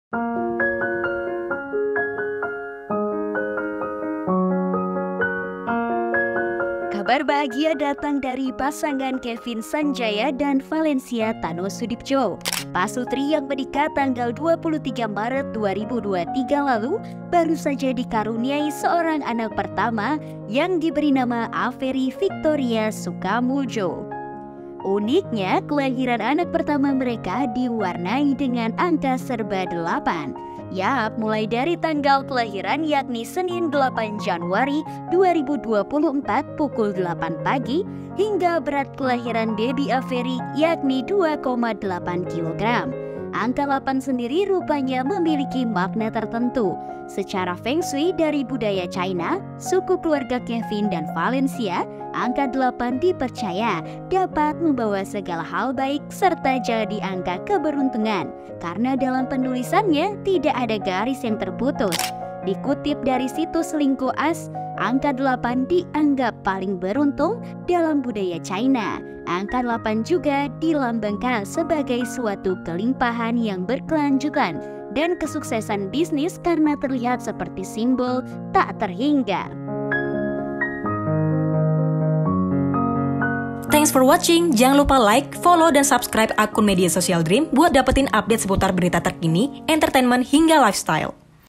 Serba Angka 8, Arti Hoki Kelahiran Anak Kevin Sanjaya dan Valencia Tanoesoedibjo
Kabar bahagia datang dari Pasangan Kevin Sanjaya dan Valencia Tanoesoedibjo.

Pasutri yang menikah tanggal 23 Maret 2023 lalu ini baru saja dikaruniai anak pertama yang diberi nama Avery Victoria Sukamuljo.

Uniknya, kelahiran anak pertama mereka diwarnai dengan angka serba depalan.

Yup, mulai dari tanggal kelahiran yakni Senin 8 Januari 2024 pukul 8 pagi.

Hingga berat kelahiran baby Avery yakni 2,8 Kg.